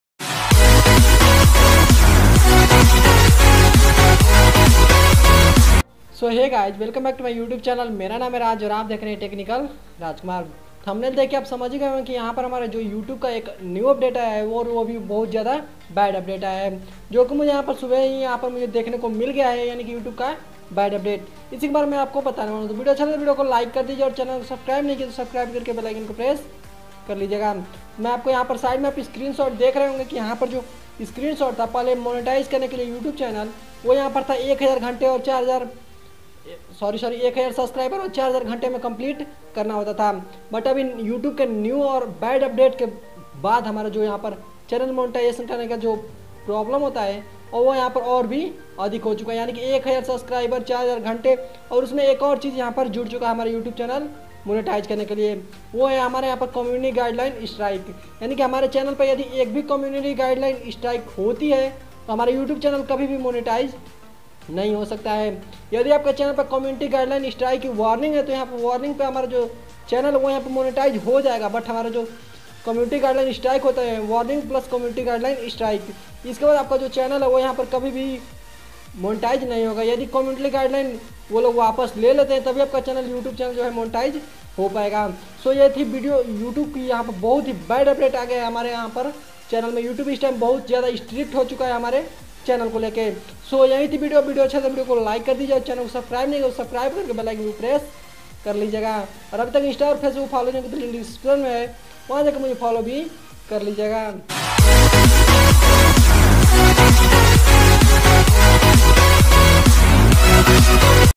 Youtube Very Bad Update 😡 ! New Monetization Update 2021
Bad Youtube Monetization Update || Ab channel monetise hone me aur jyada problem

Request ...🙏🙏🙏...